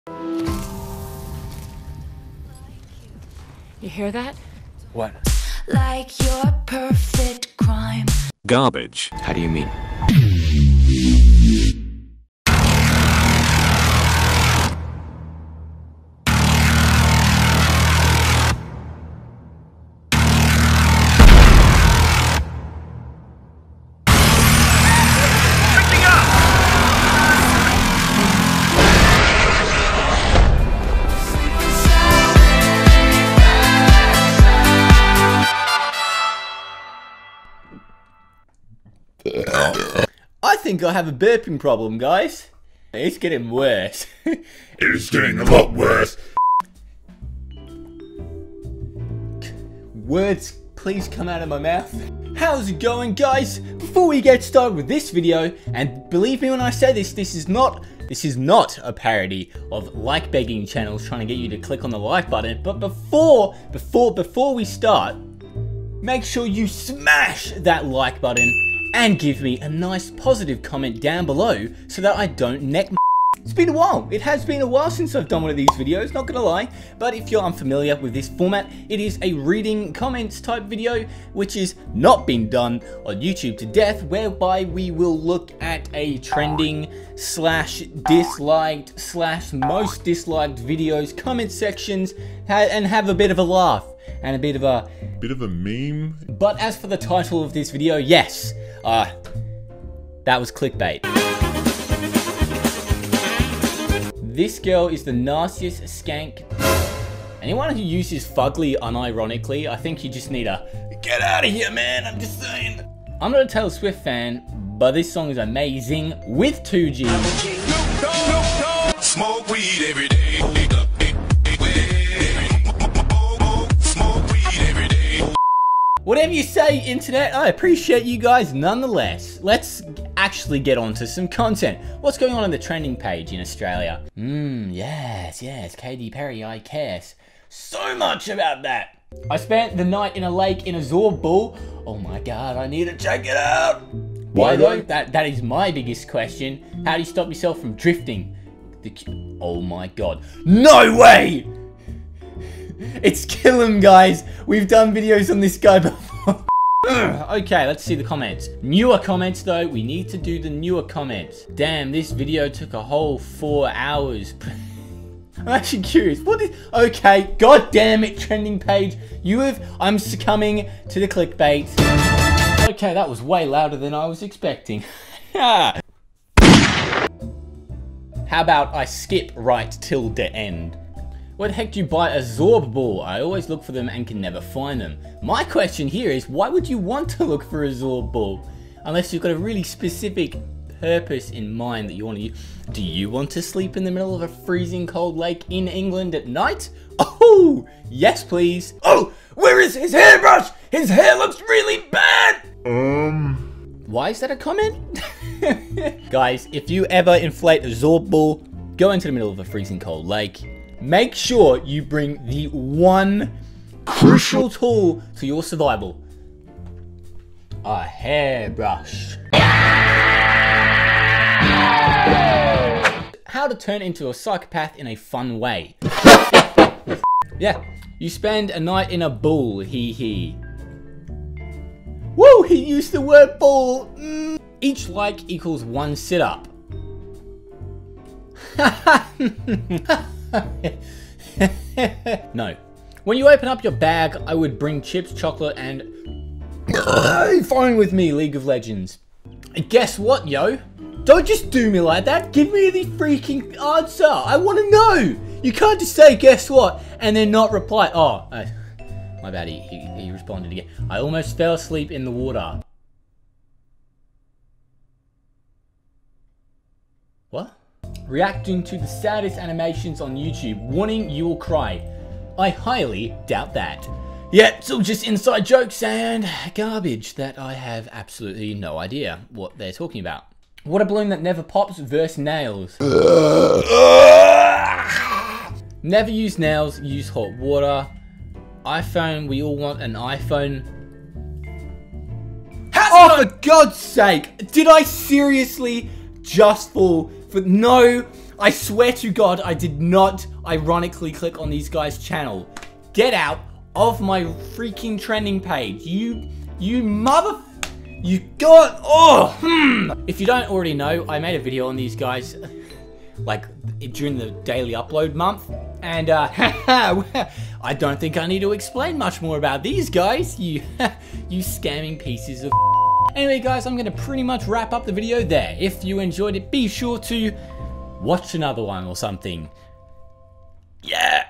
Taylor Swift QUITS music to become a Xenomorph
Sooo yeah, pretty much just hopping on that 'trending' train for views. Haven't done an episode of the trending comments series in a while so it feels good getting completely sidetracked for no reason whatsoever!
~ Ruppy